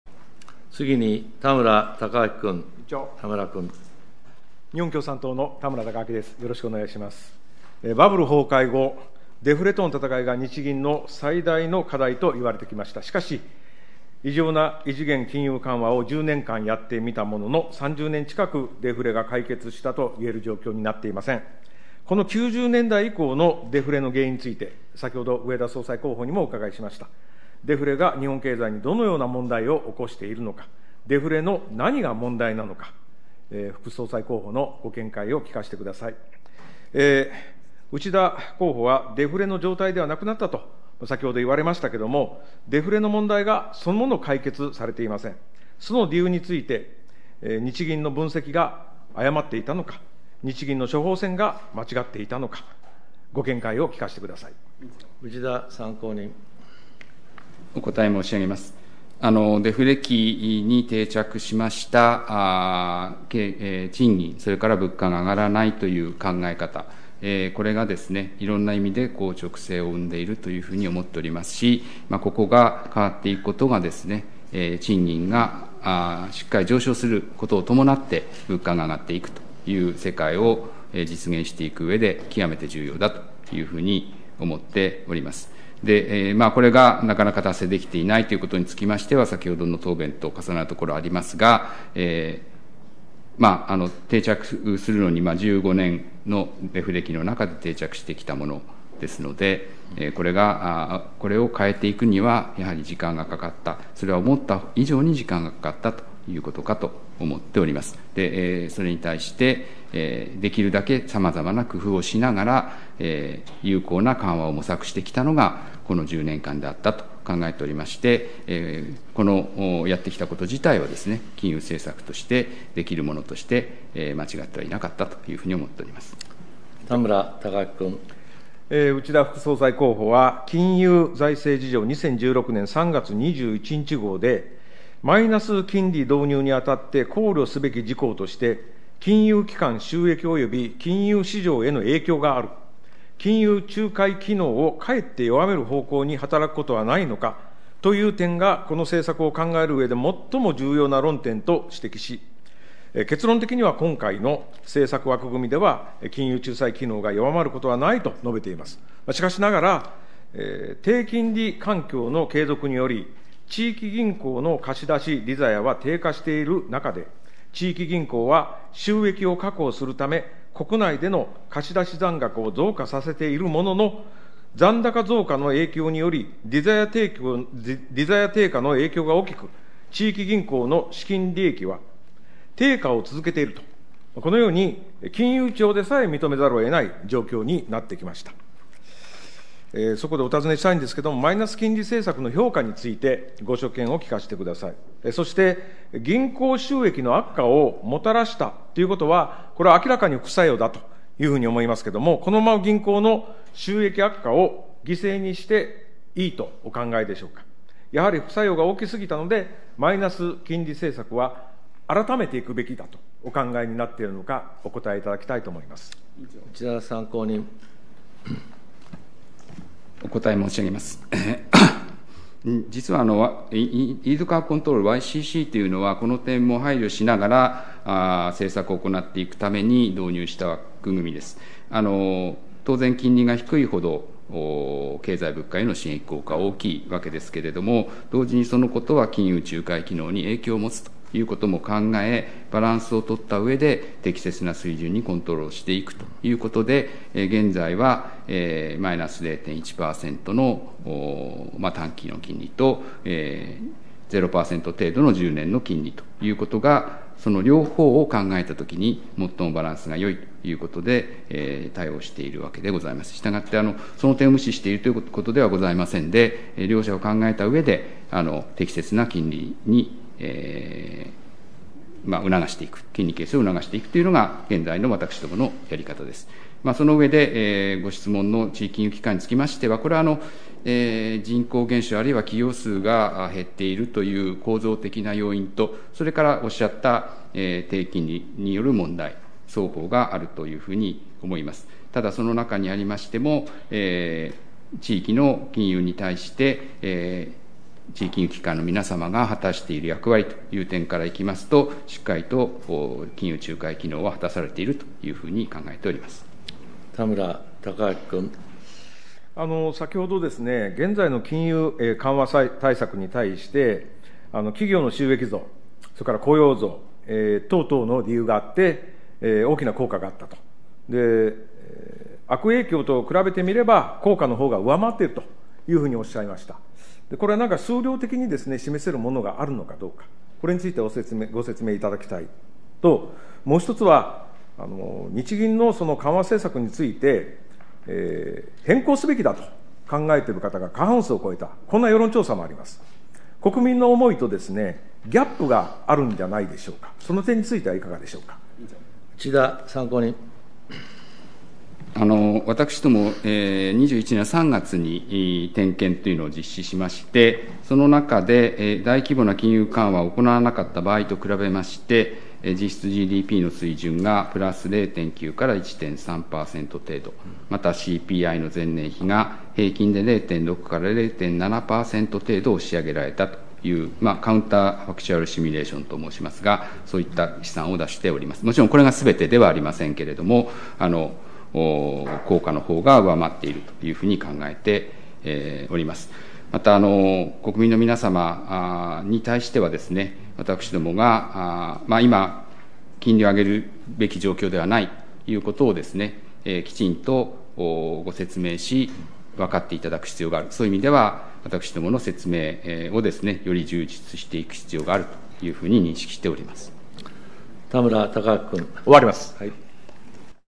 内田氏、日銀の低金利政策を評価 2023.2.24
議院運営委員会 田村貴昭 衆院議員質問　参考人 内田眞一 日本銀行副総裁候補者（日銀理事）